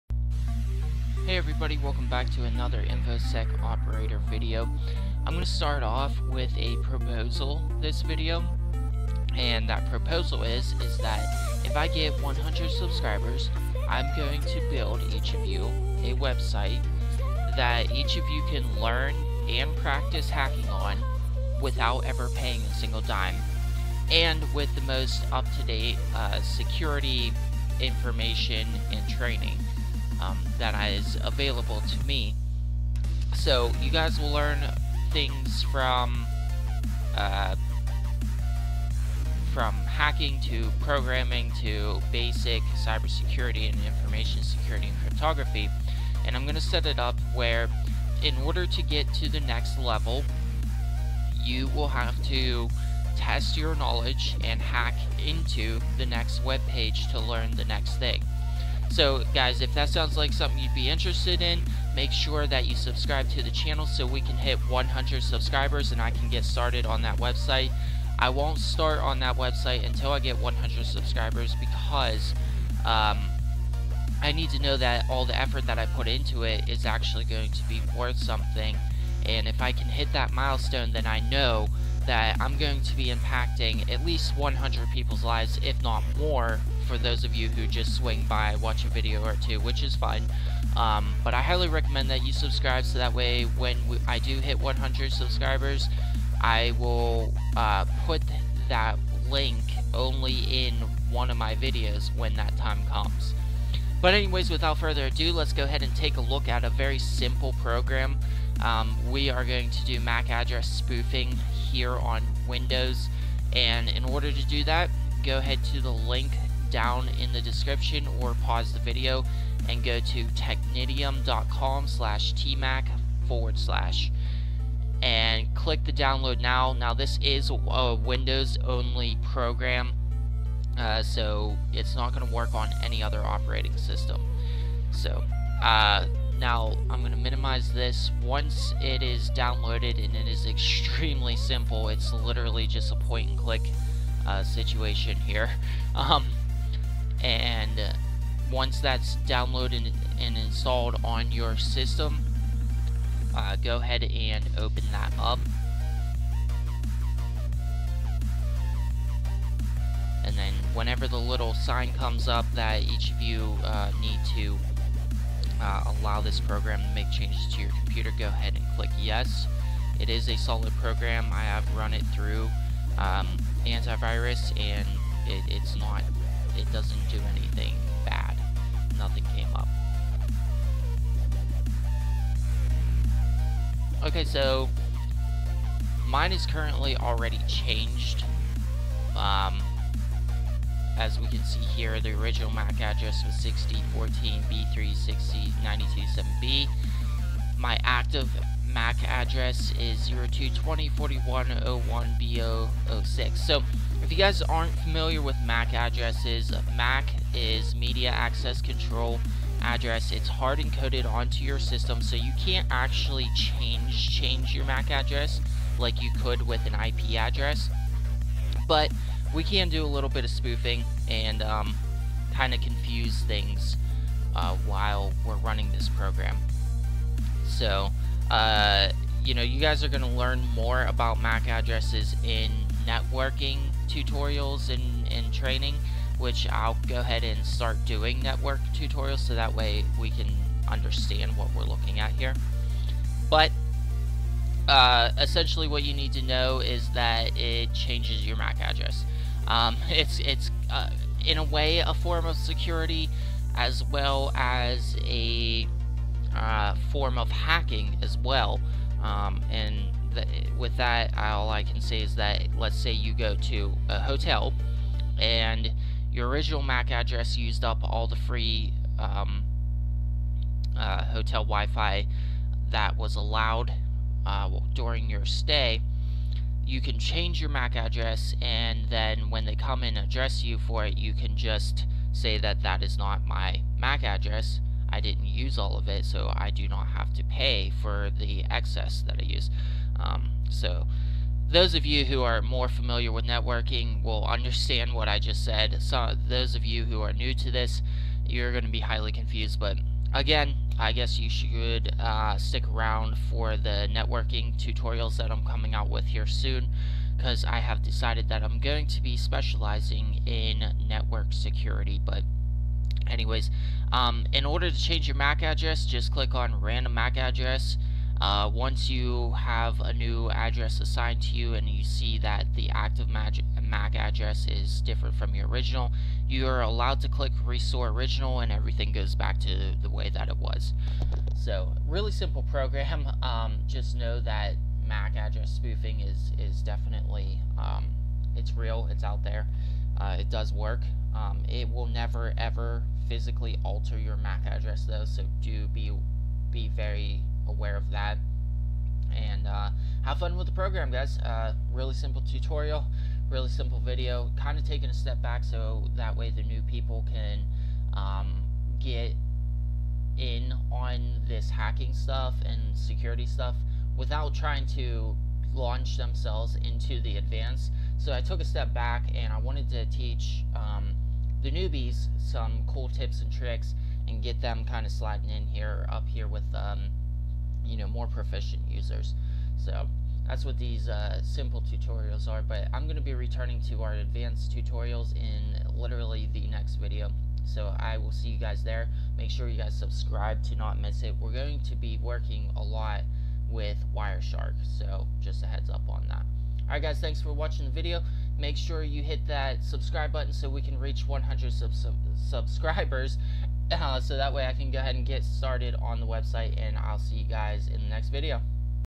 Windows MAC Address Spoofing - Change Your MAC Address on Windows OS
Ever wonder how to do MAC Address changing on Windows Operating System(s)? In this video we take a look at a simple program called TMAC which enables you to alter your MAC (Media Access Control) Address on the Windows Operating System, providing a temporary layer of security on your Network(s).

I do not condone the use of this program for illegal purposes. I am not responsible for the misuse of this information for illegal activities, as this tutorial is only for educational and ethical purposes only.